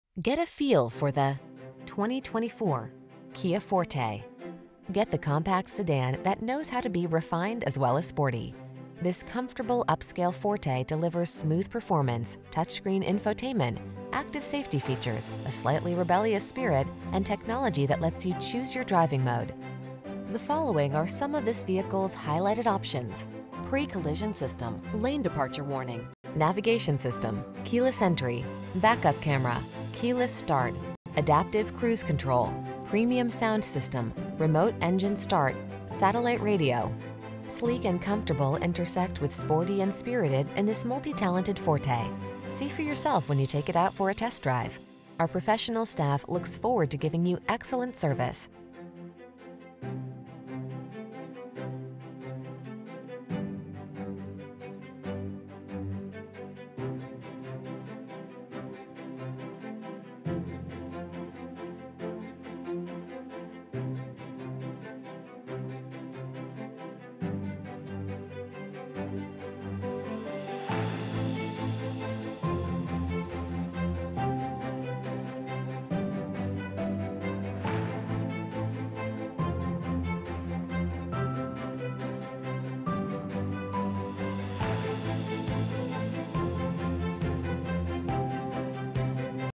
2024 Kia Forte Gilbert, Chandler, Mesa, Queen Creek, San Tan, AZ 240664
Aurora Black New 2024 Kia Forte GT-Line available in Gilbert, Arizona at Horne Kia. Servicing the Chandler, Mesa, Queen Creek, San Tan, AZ area.

Horne Kia
1465 E Motorplex Loop